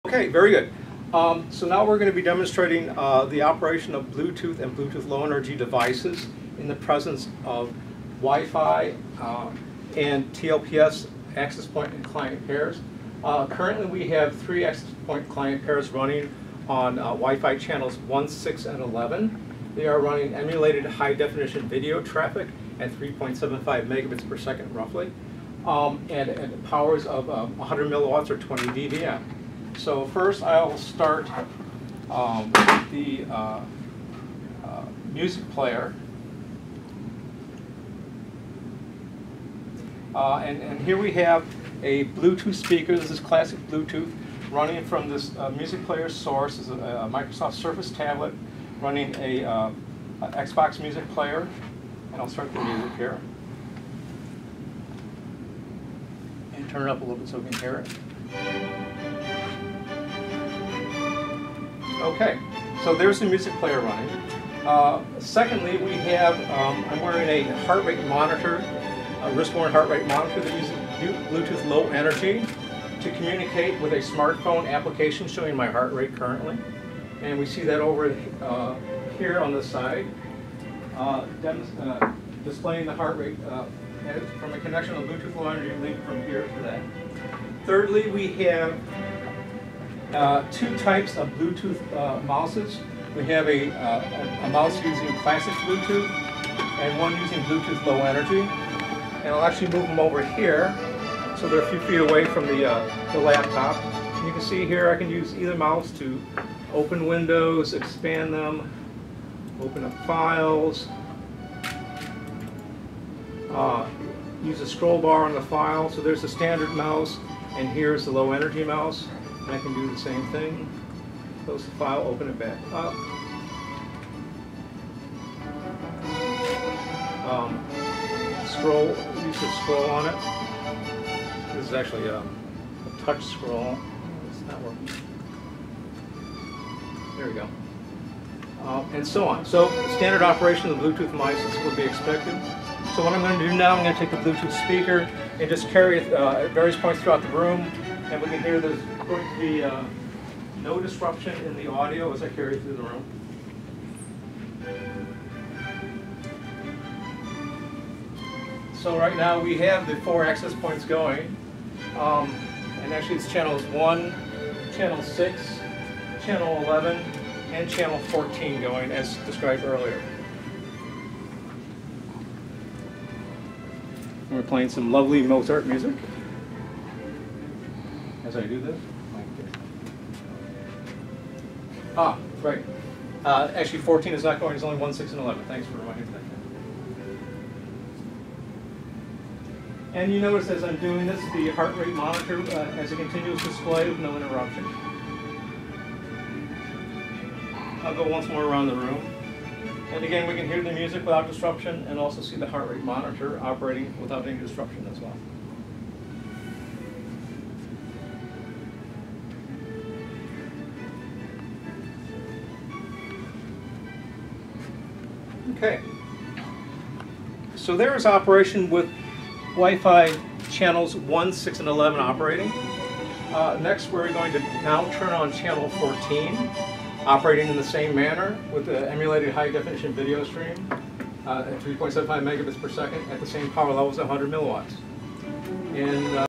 Demonstration of Bluetooth devices operating with Channels 1, 6 and 11
Mike Needham with Roberson & Associates demonstrates Bluetooth devices operating with Channels 1, 6 and 11

These videos are of a reenactment of Globalstar’s March 6, 2015 Bluetooth demonstration to the FCC at its Technology Experience Center. This reenactment occurred on March 10, 2015.